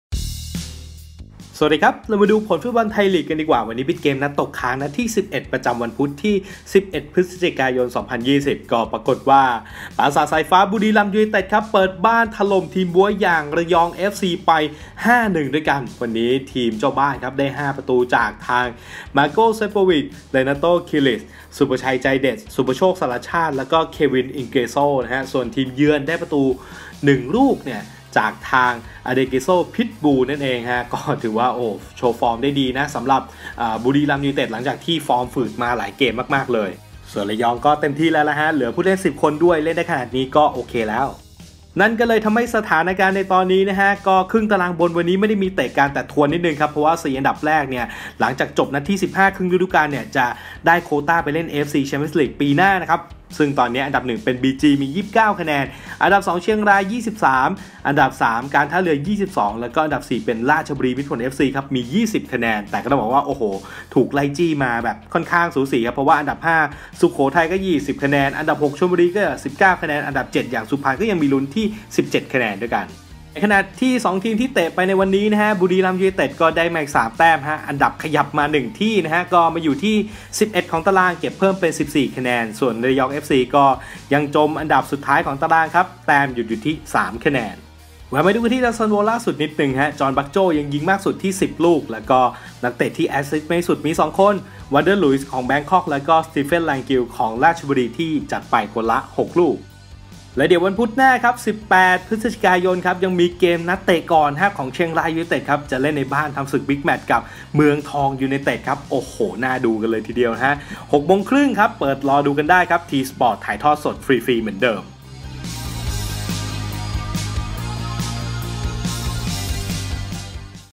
ผลบอลไทยลีกล่าสุด นัดตกค้าง : บุรีรัมย์ไล่ขยี้ระยองกระจุย ปลดล๊อกได้หลังฝืดมาหลายนัด(11 Nov 2020)
สำหรับไฮไลต์สามารถเข้าไปดูได้ที่ Thai League Channel ตามลิ้งด้านล่างครับ

1.บุรีรัมย์ ยูไนเต็ด vs ระยอง เอฟซี

Timestamp
0:00 Intro
0:03 ผลบอลไทยลีกล่าสุด
0:42 ตารางคะแนนไทยลีกล่าสุด
1:29 อันดับดาวซัลโว
1:41 โปรแกรมไทยลีกนัดต่อไป